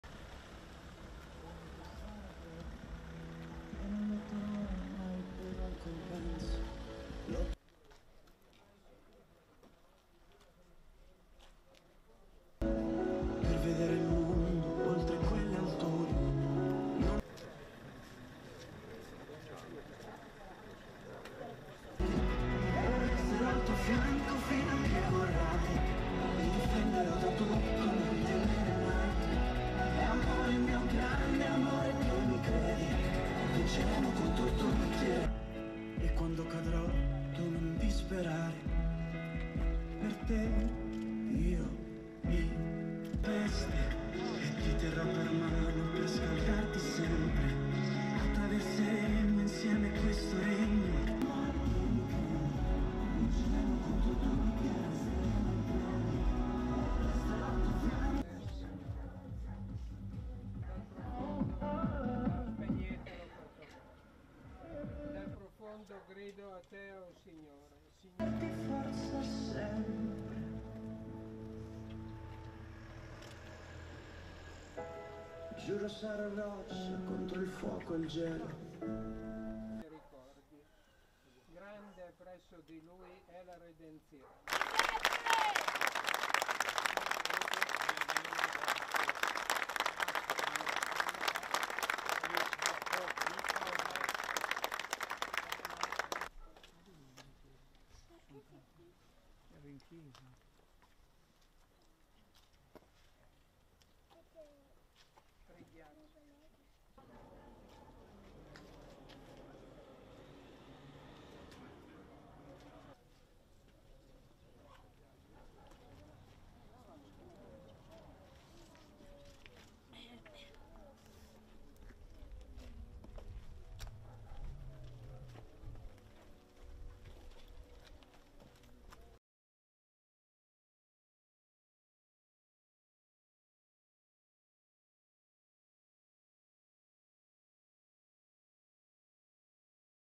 Strage di Brandizzo, a Vercelli i funerali di Giuseppe Saverio Lombardo, una delle cinque vittime
Il feretro di Giuseppe Saverio Lombardo arriva alla parrocchia Parrocchia di Sant'Antonio da Padova di Vercelli per i funerali. L'uomo è una delle cinque vittime del disastro ferroviario di Brandizzo.